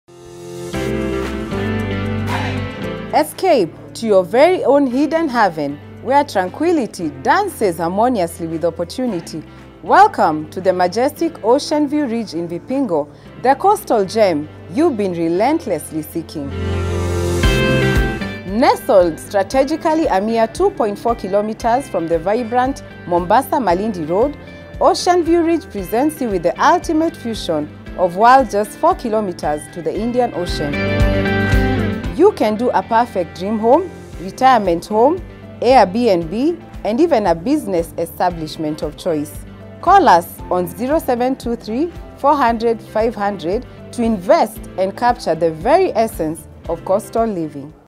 Experience the beauty of Coastal Living unraveled at Vipingo
Ocean View Ridge - Vipingo by Optiven is the epitome of refined coastal living located just a stone's throw away from the vibrant Mombasa-Malindi Road.

This mesmerizing project beckons those with discerning tastes and ambitious aspirations to build their dream homes in an awe-inspiring coastal region.